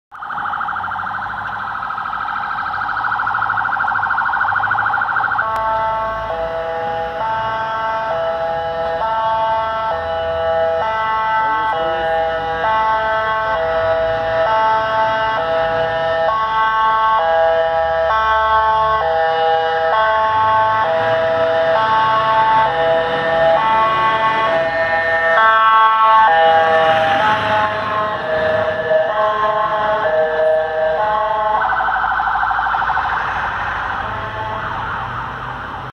Ambulancia 05 de Medical
Bonito Fler que se forma con la luz que emiten los códigos y torreta